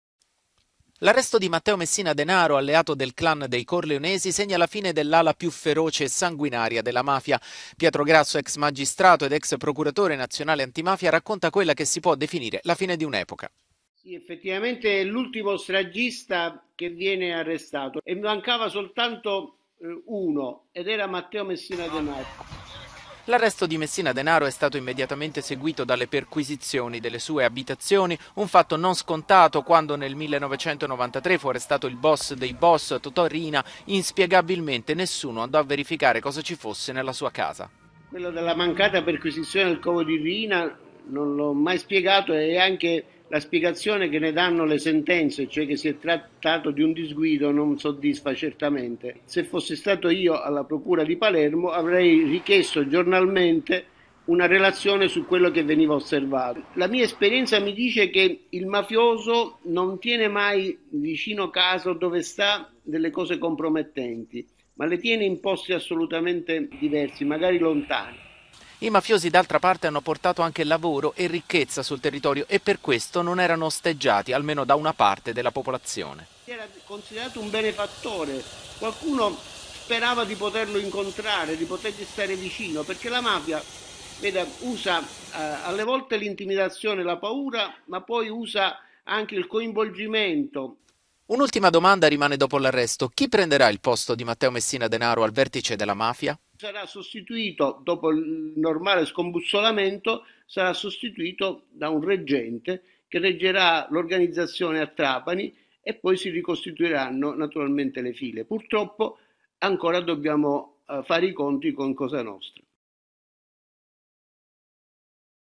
Pietro Grasso: "L'arresto di Messina Denaro non è la fine della lotta alla mafia"
L'ex procuratore nazionale antimafia avverte che nonostante l'arresto del boss la mafia si riorganizzerà